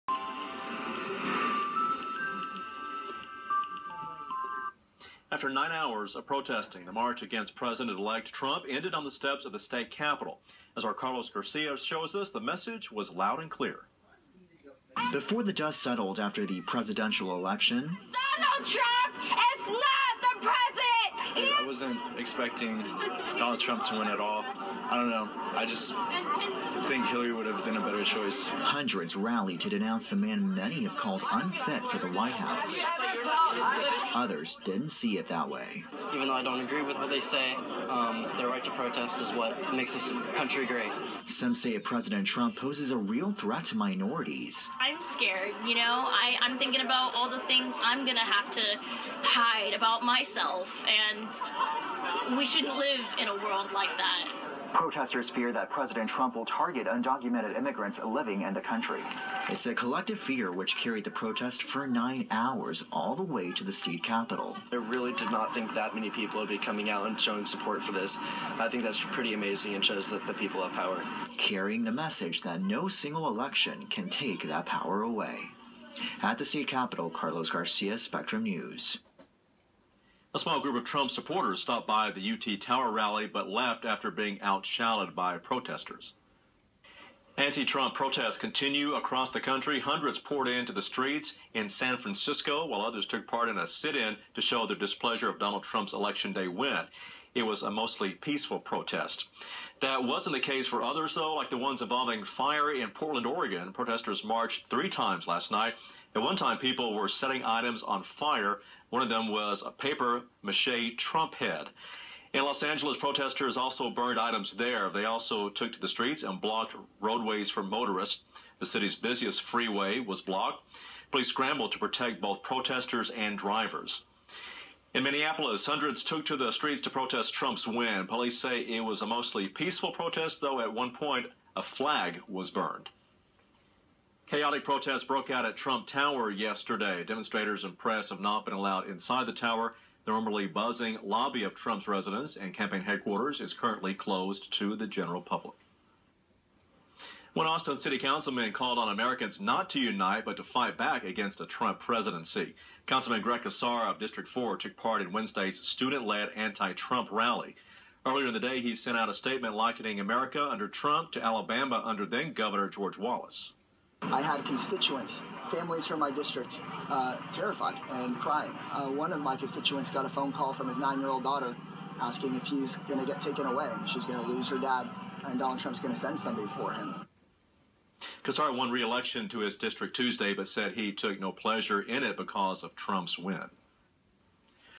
Donald Trump Protests news report. (Spectrum news, Austin TX)
From Spectrum news in Austin Texas. Recorded Thursday 20161110. Next few months will be interesting.